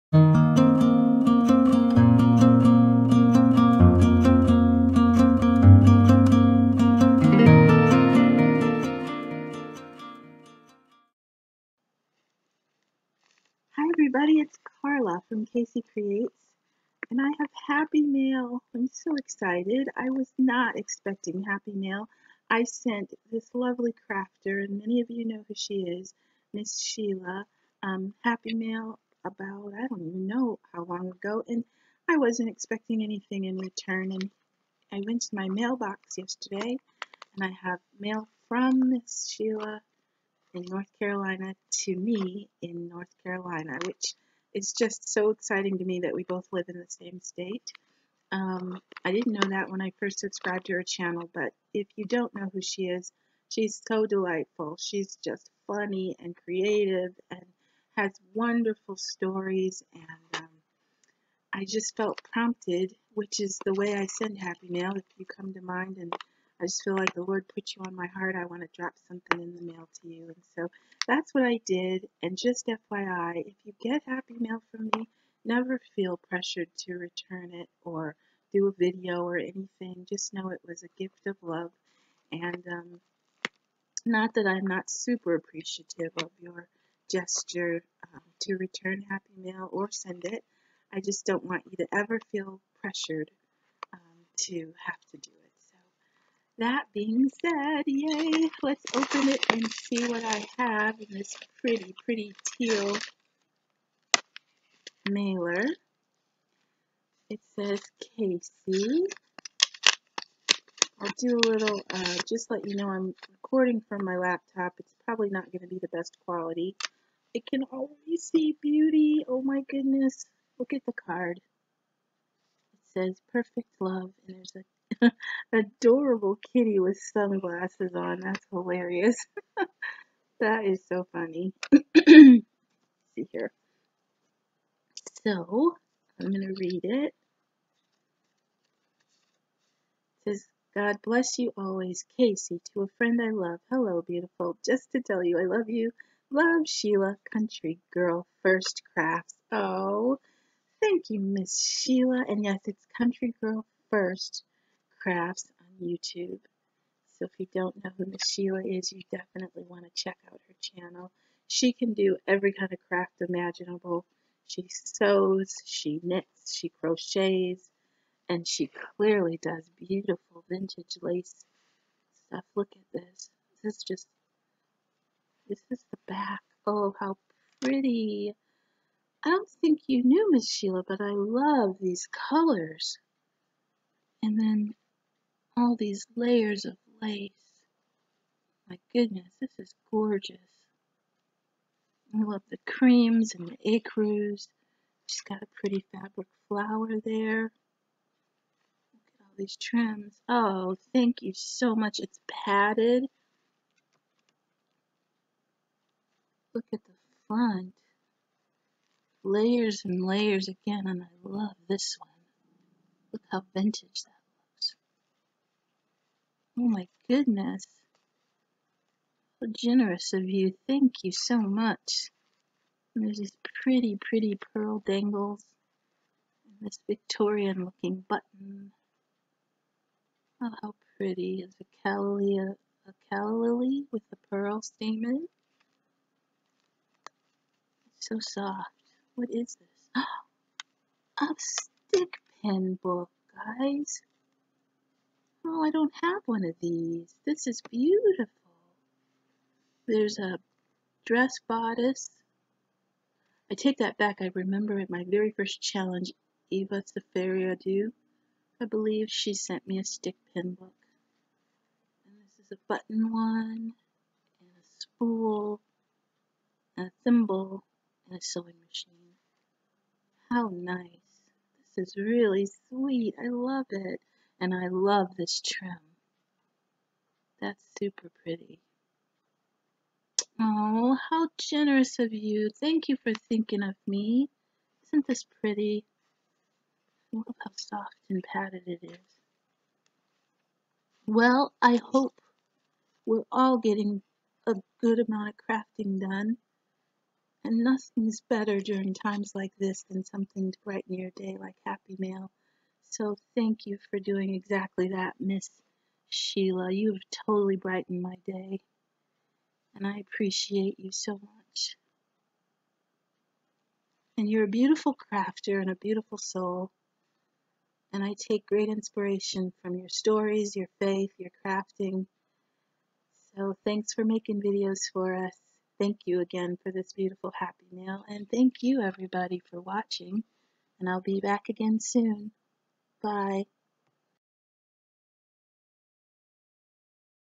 Happy Mail from Country Girl First Crafts - Ms. Sheila
I received the prettiest hand crafted needle book! I wanted to show you and tell you that if you don't know Ms Sheila you'll want to go over and visit her channel. She's creative, talented, funny and a joy to watch!  Thank you NC neighbor you're the best! xx